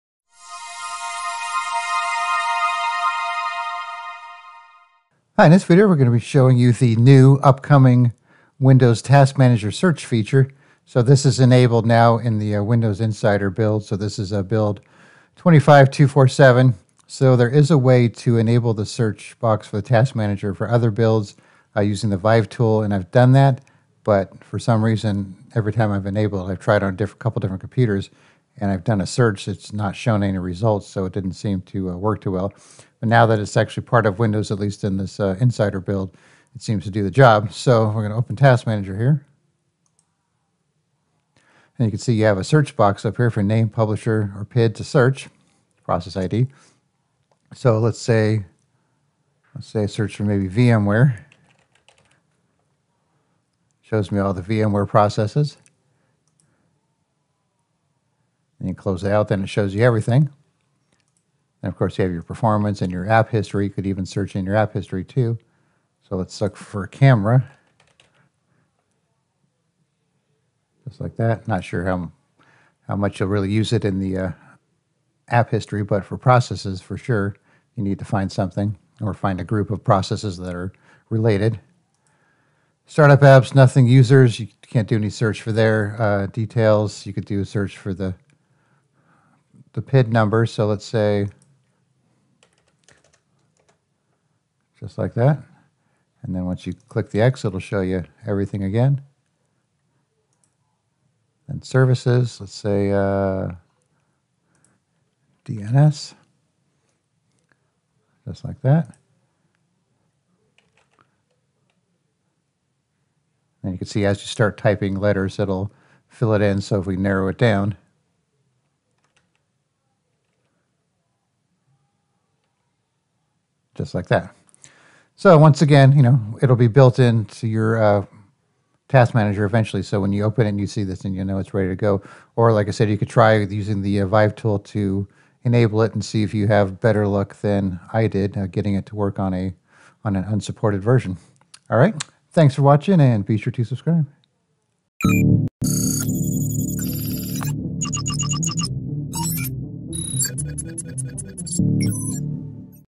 The Windows Task Manager Search Box
The Windows Task Manager now features a search bar or box where you can search for things such as process names, PIDs, publisher names, service names, recently used apps and so on. This feature will be available in the retail version of Windows 11 but for now it's only available for the Windows Insider builds of Windows 11.